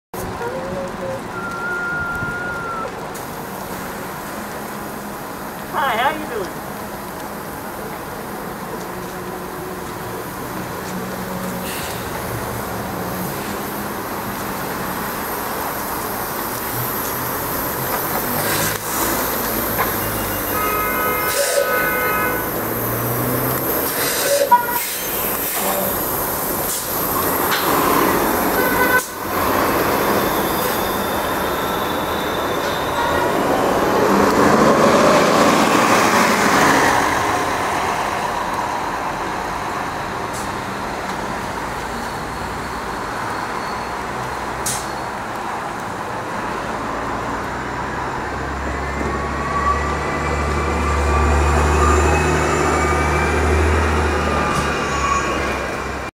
𝟕𝟐𝟎𝐩⁶⁰ 𝐍𝐞𝐰 𝐅𝐥𝐲𝐞𝐫 𝐗𝐜𝐞𝐥𝐬𝐢𝐨𝐫 𝐗𝐃𝟒𝟎 𝟕𝟕𝟐𝟏 𝐐𝟓𝟖 𝐁𝐮𝐬 @ 𝐏𝐮𝐭𝐧𝐚𝐦/𝐅𝐚𝐢𝐫𝐯𝐢𝐞𝐰 𝐀𝐯𝐞𝐧𝐮𝐞𝐬
Here in action is the New Flyer Xcelsior XD40 7721 on the Q58 route at Putnam and Fairview Avenues

Train Transfer: M train

Thankx for watchin' and stick around for the latest uploads